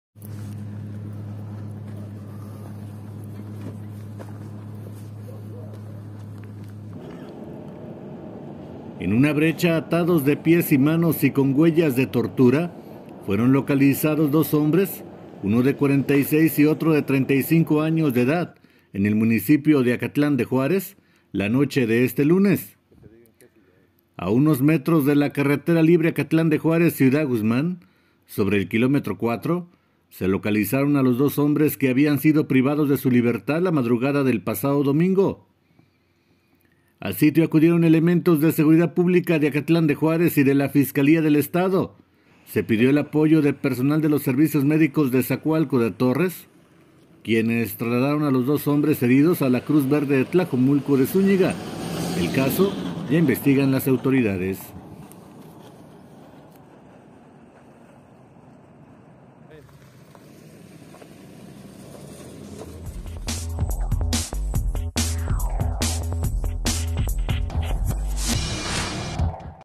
Localizan dos masculinos maniatados en Acatlánbde Juárez.
Seguridad | Acatlán.- En una brecha en el tramo carretero Acatlán-Zacoalco, fueron localizados dos hombres maniatados la noche de este lunes.